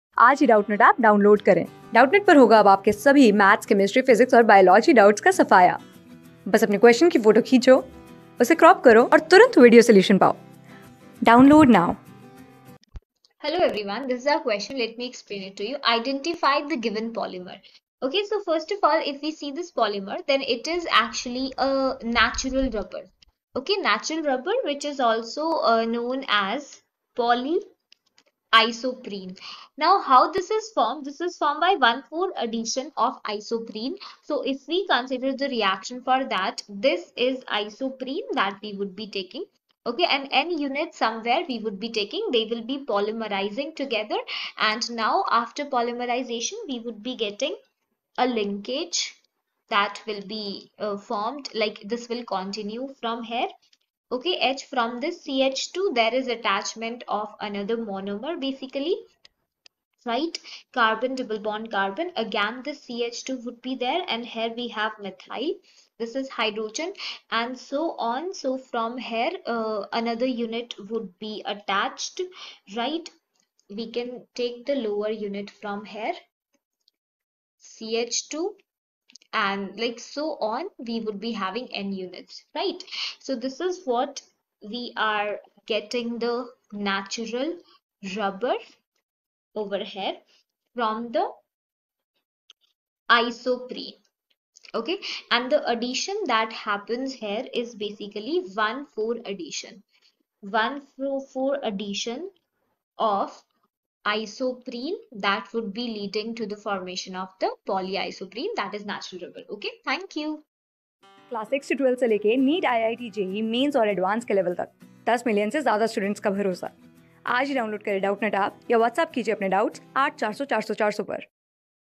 Identify the polymer given below | CLASS 12 | CHEMISTRY | Doubtnut
Identify the polymer given below
Class: 12
Subject: CHEMISTRY
Chapter: POLYMER
Board:CBSE

👉 Have Any Query? Ask Us.